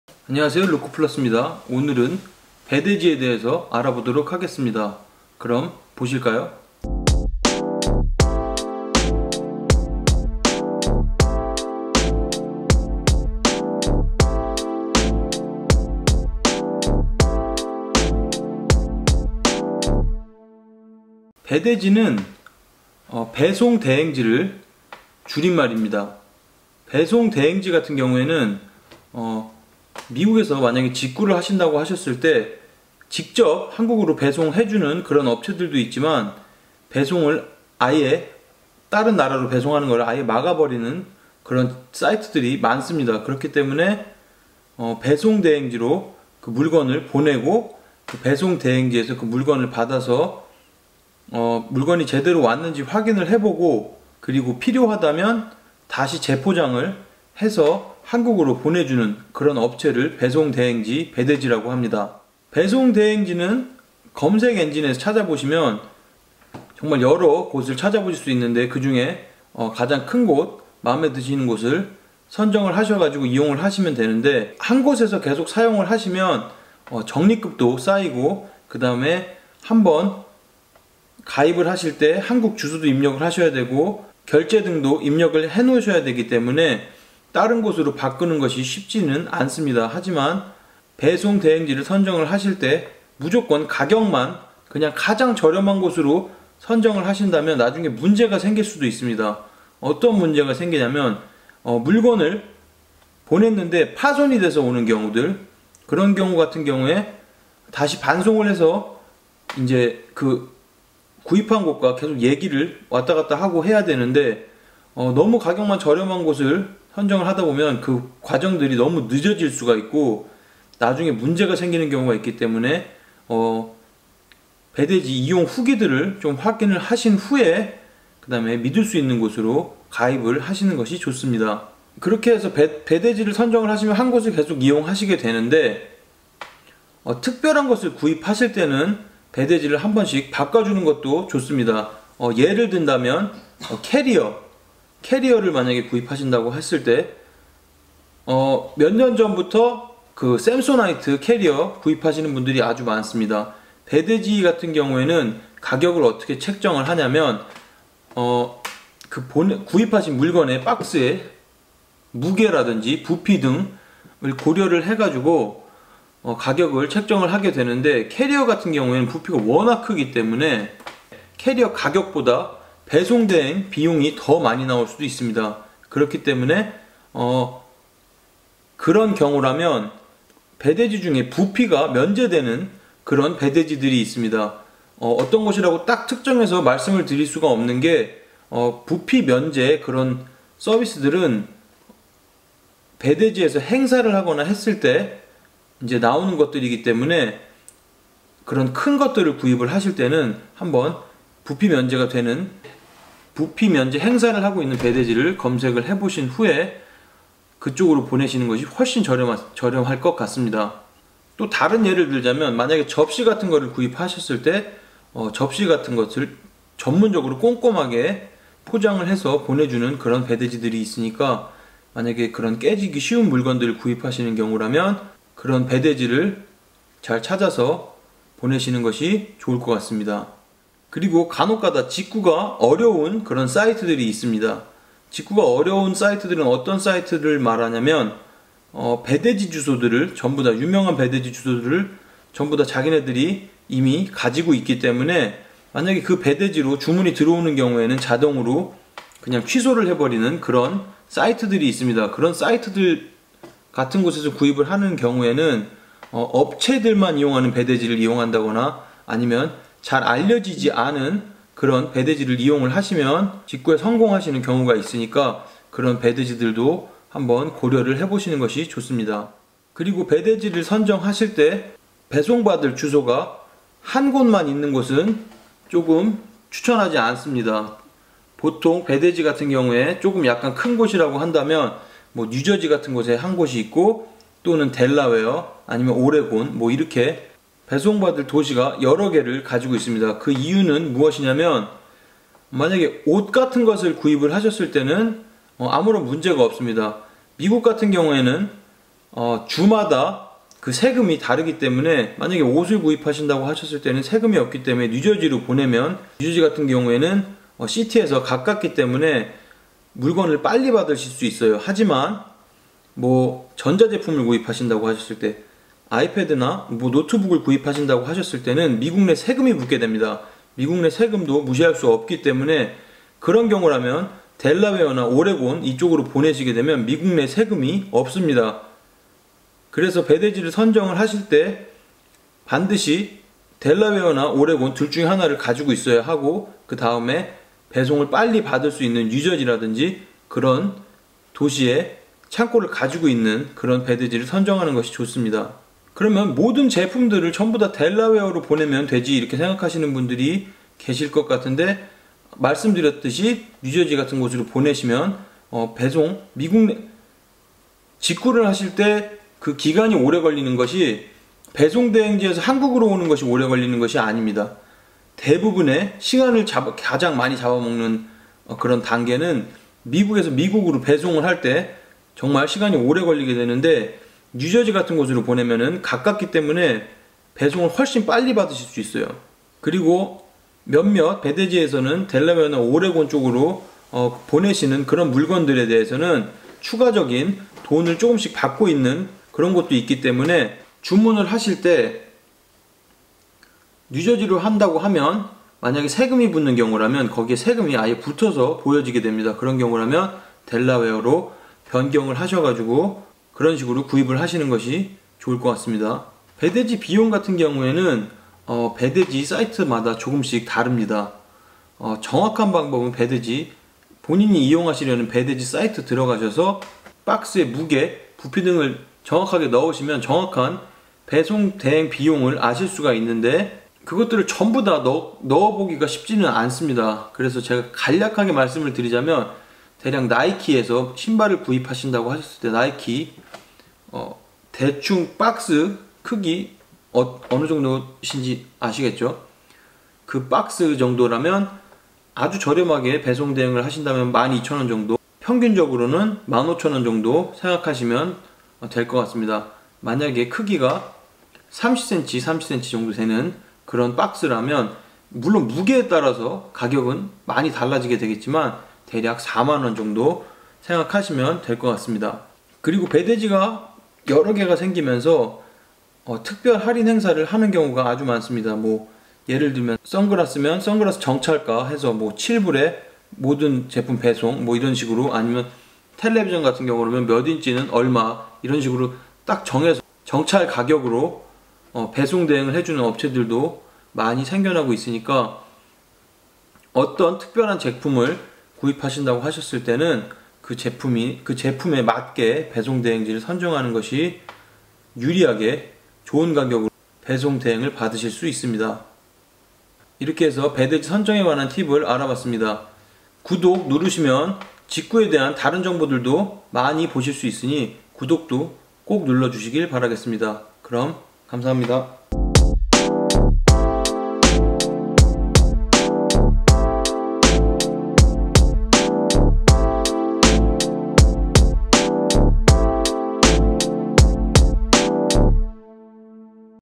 배송대행지 어떻게 골라야 할까? 배대지 선정 방법
직구를 하려면 꼭 알아야 할 배대지 선정 방법!! 어떤 배송대행지를 골라야 나중에 낭패를 보지 않을까요?

부피가 큰 제품을 구입했다면? 깨지기 쉬운 제품을 구입했다면 등등 배대지 고르는 방법을 알려 드립니다.

감사합니다.